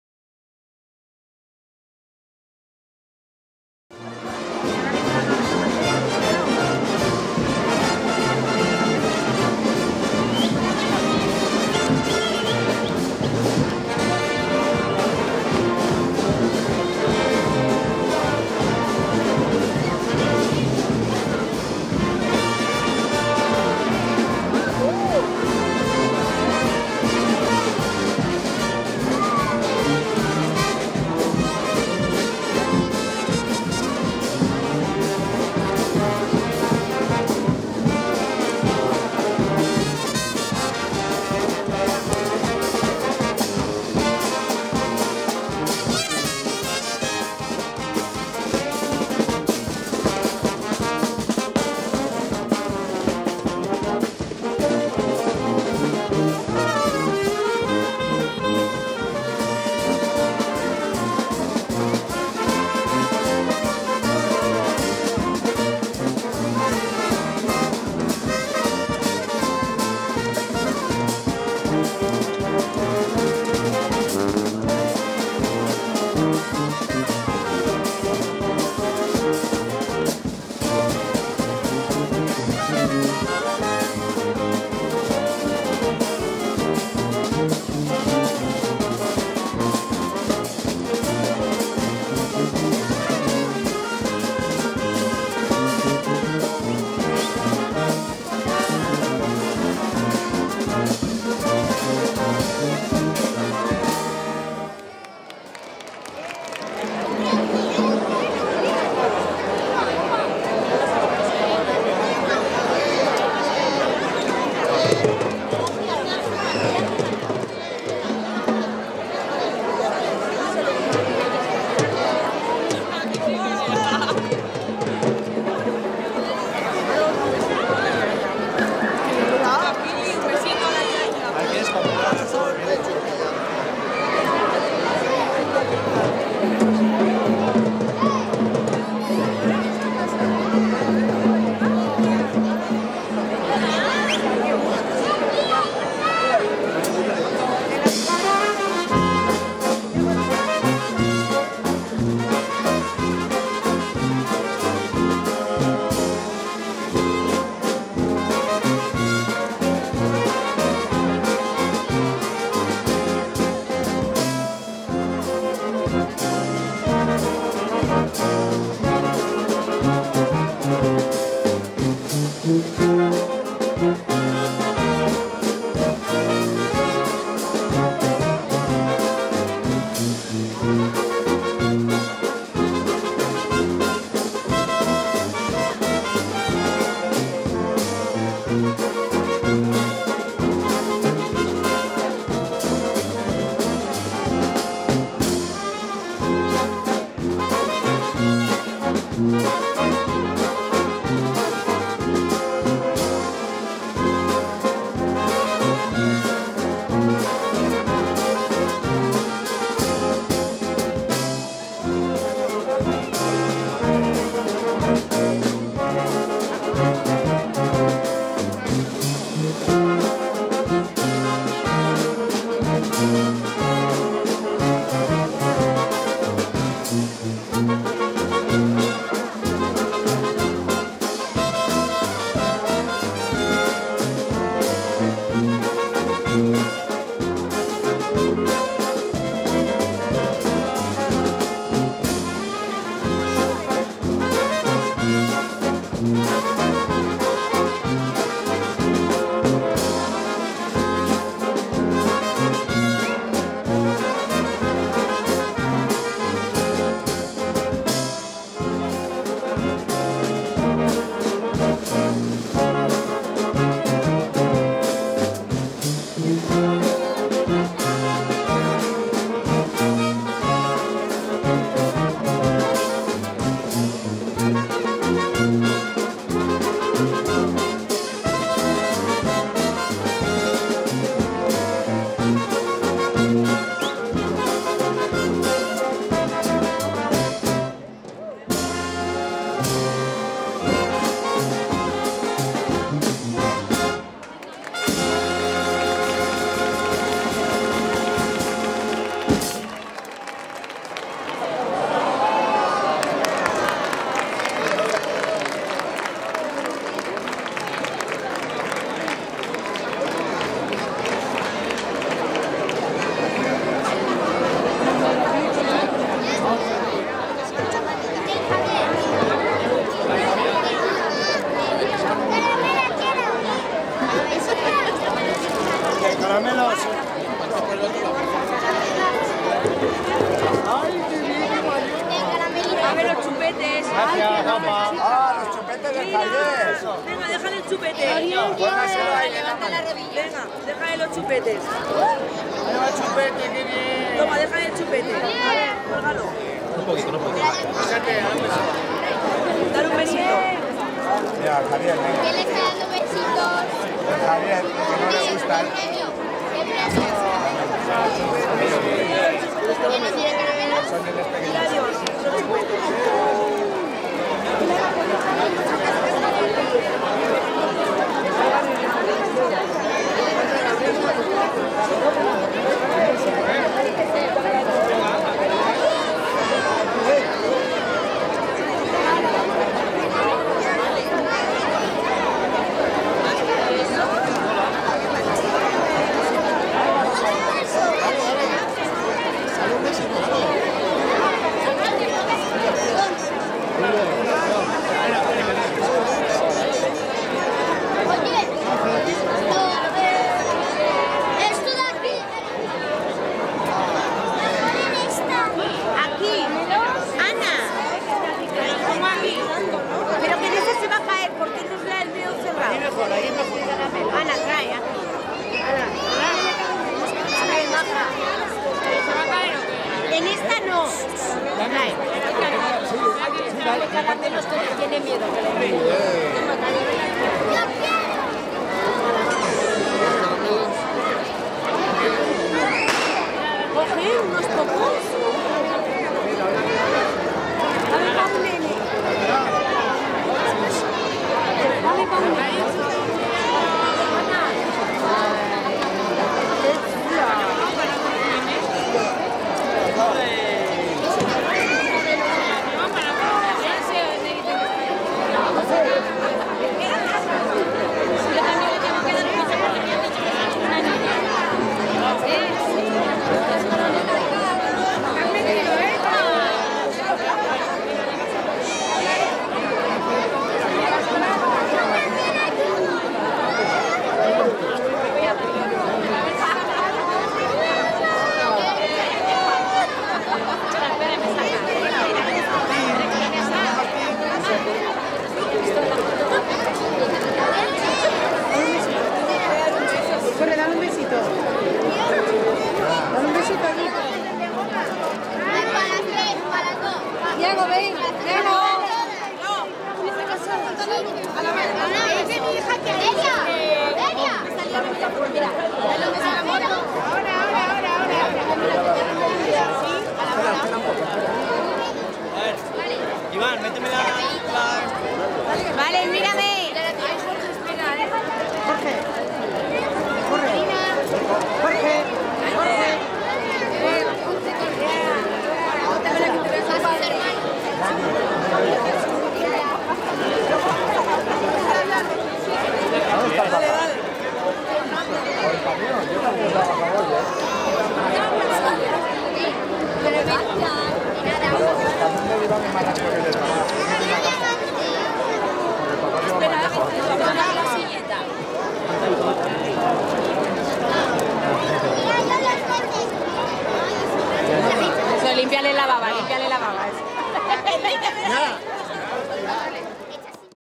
Tv Azagra - 14/09/2022 - Fiestas - Despedida de los Gigantes.
Despedida a la comparsa de Gigantes y Cabezudos.